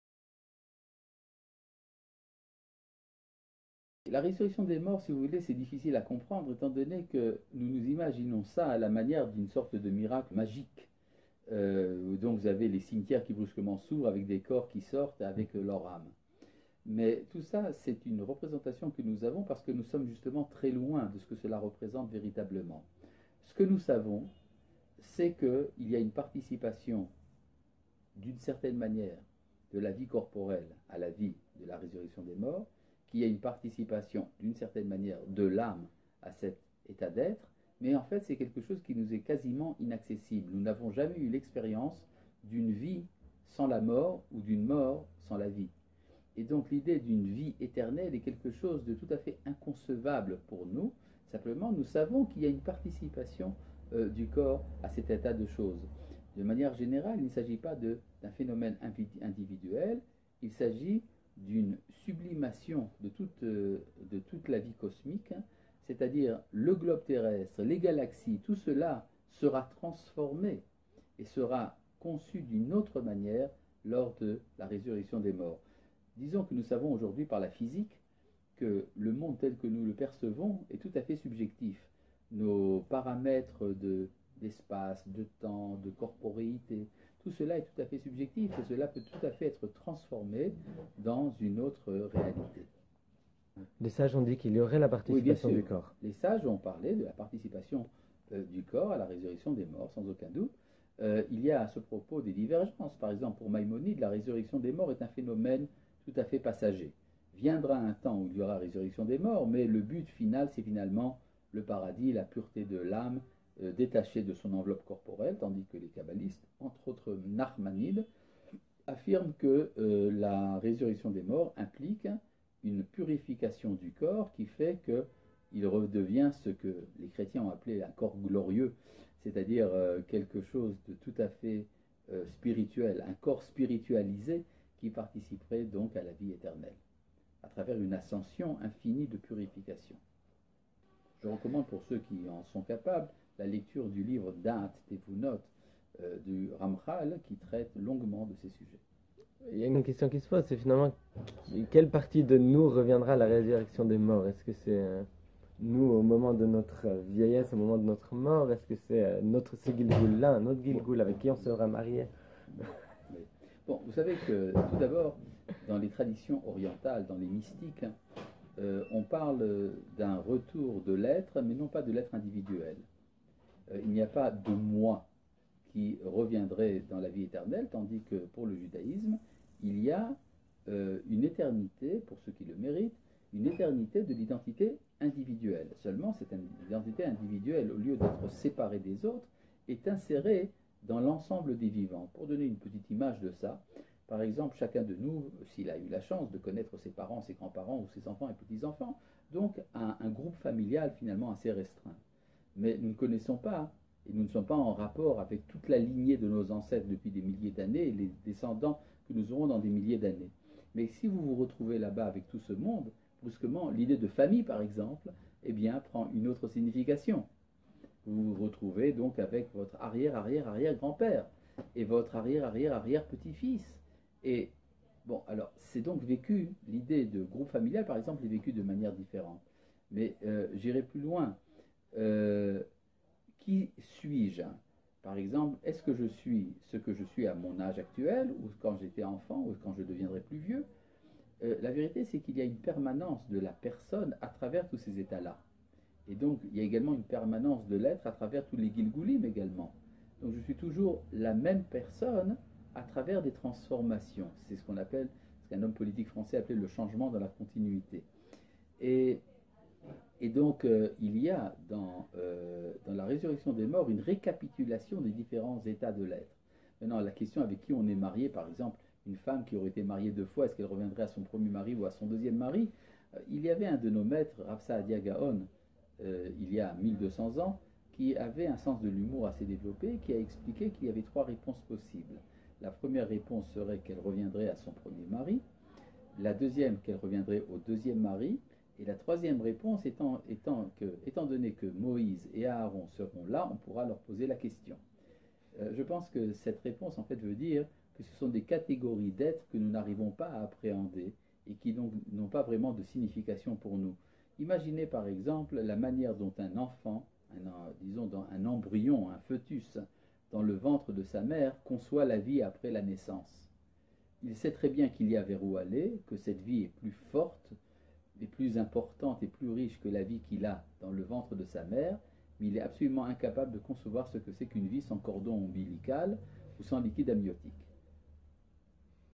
Résurrection des morts
Que se passera-t-il lors de la résurrection des morts?
Qui sera ressuscité?
Serais-je ressuscité enfant, adulte, vieillard?
Avec qui serais-je marié?

Désolé pour la qualité du son, il faut mettre le volume à fond...

Soutenez le Projet Noachide et ensemble nous allons rendre le monde meilleur